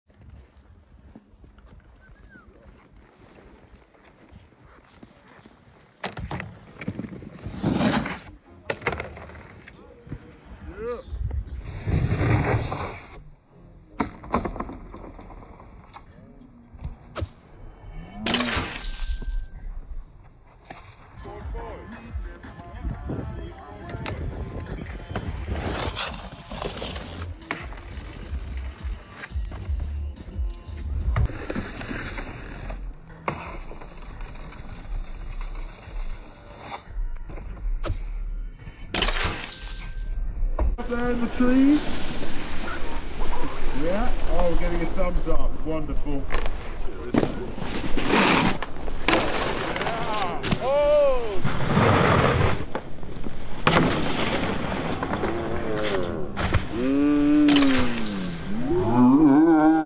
Spring Battle 23 | Best Rail - Snowboard Women
BEST RAIL AWARD - Snowboard Women
1. Anne-Sophie Lechon (BEL)
2. Selin Lakatha (AUT)
3. Melissa Pepperkamp (NED)